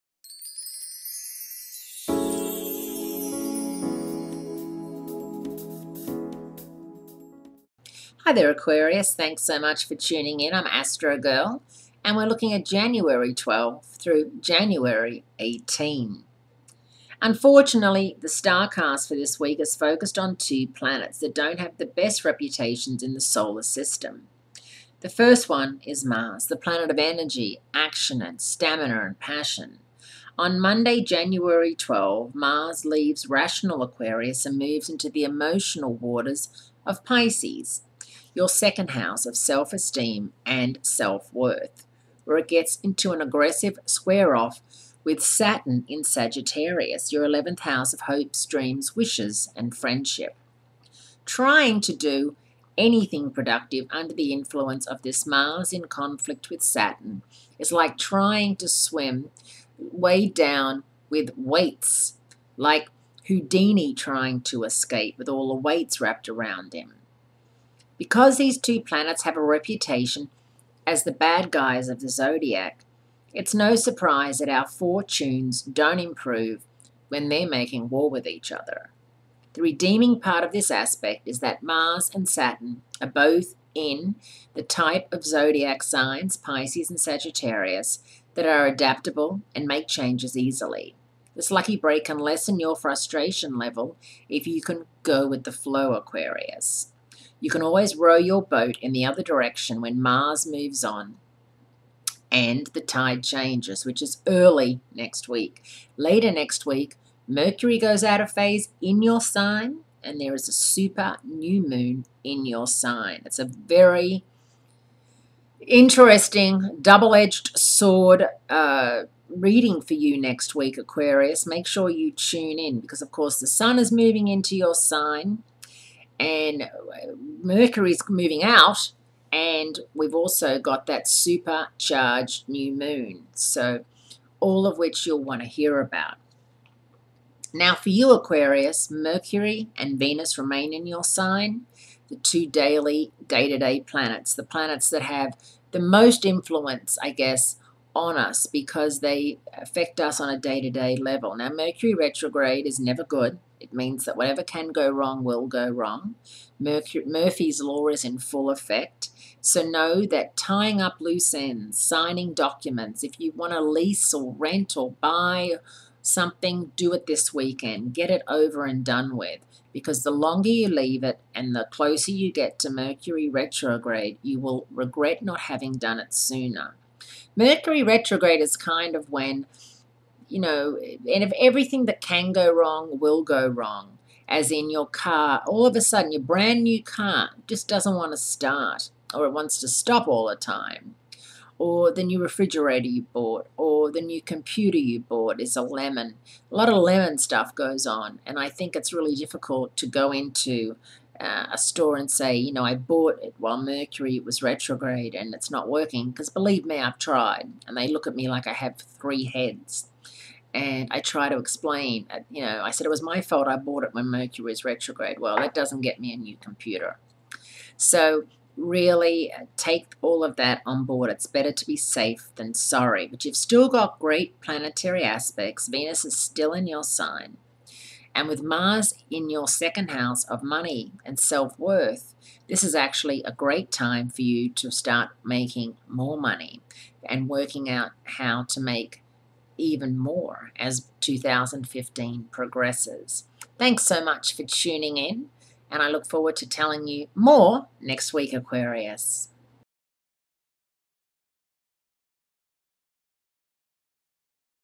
Astrogirl – Aquarius – 12 January 2015, Weekly Horoscopes
Horoscope Highlights for Aquarius in 2015 - After August 11, your love life will heat up with Jupiter the planet of opportunity moving into your 8th house of transformation. Perfect matches- Leo, Gemini & Librans.